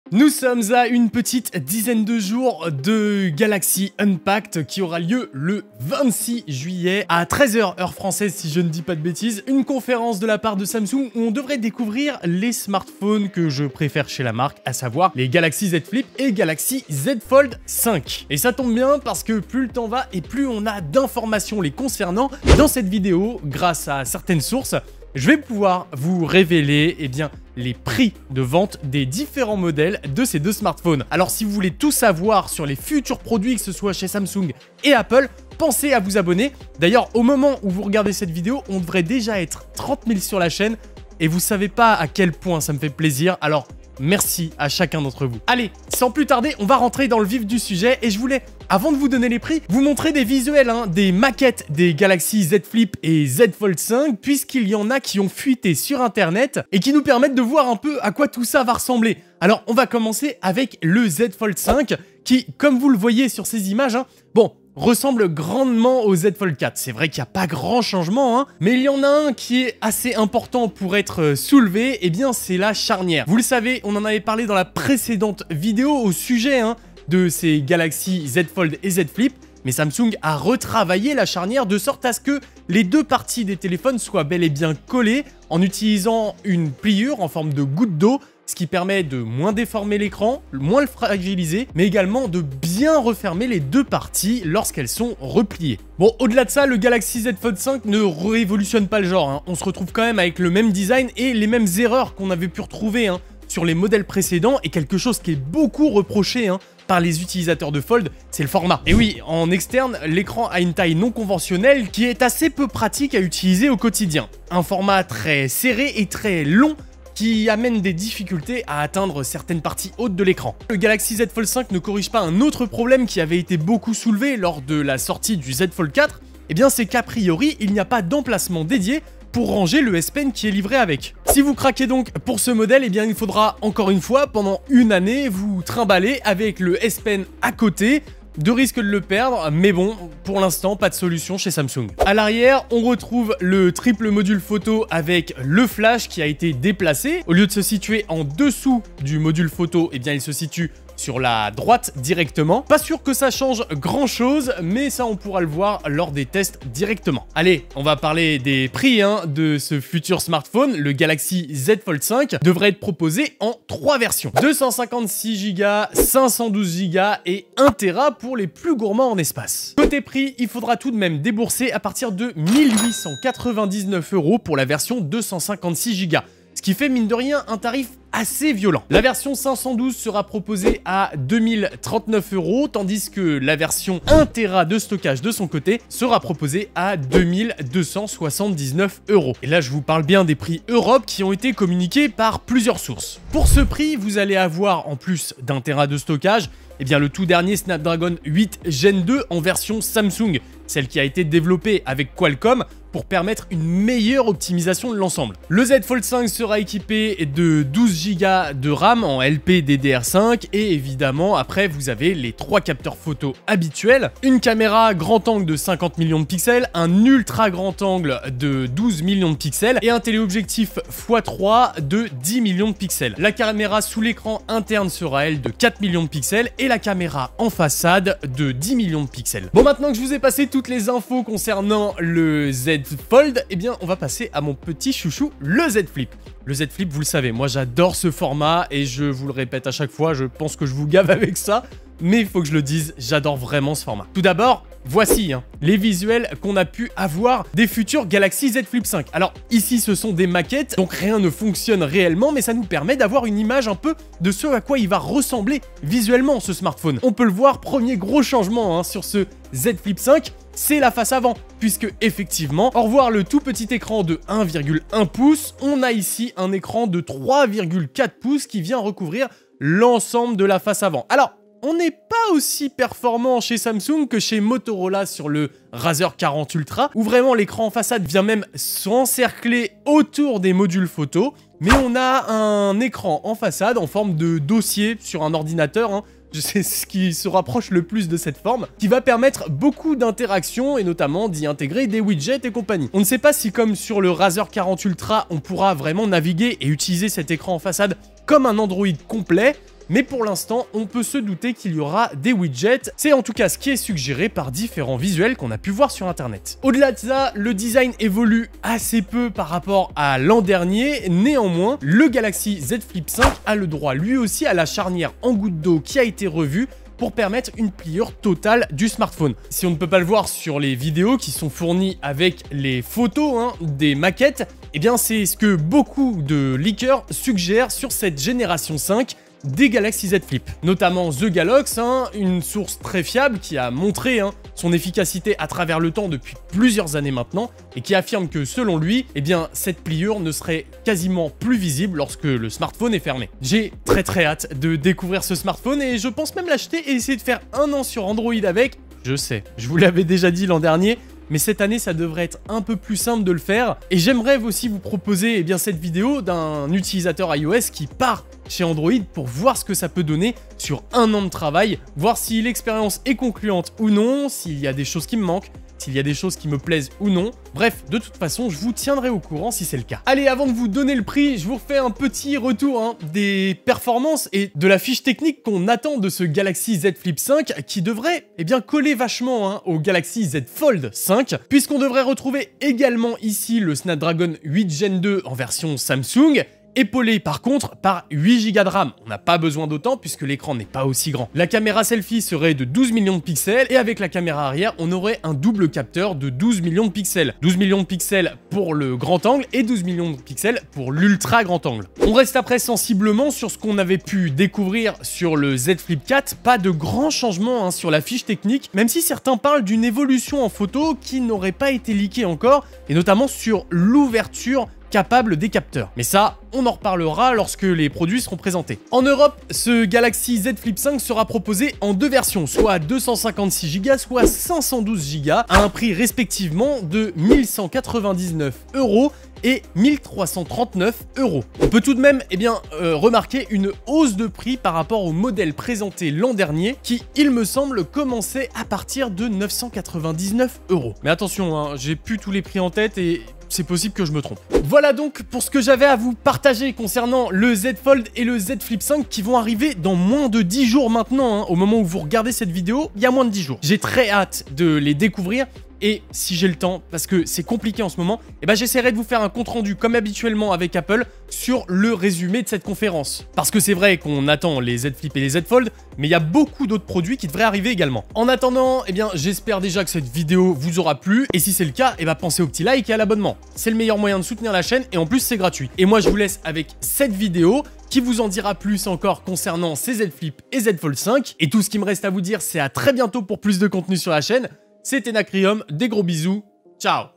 Samsung Galaxy ZFLIP 5 et Z FOLD 5 : ON CONNAIT LES PRIX !! 🤑🤑
a dix jours de la présentation des nouveaux smartphones pliants de Samsung, l'information vient de fuiter! On connait l'ensemble de la fiche technique des nouveaux Zflip 5 et Zfold 5 mais aussi et surtout leurs PRIX!

Les Galaxy Z Flip 5 et Galaxy Z Fold 5 ont, comme habituellement été totalement dévoilés avant même la présentation officielle!

Sources Visuelles:
SABRE LASER:
Bénéficiez de -10% avec le code NAKRYUM10 chez Sabre Galactique!
Les meilleurs accessoires iPhone et Samsung mais aussi les produits que je vous recommande: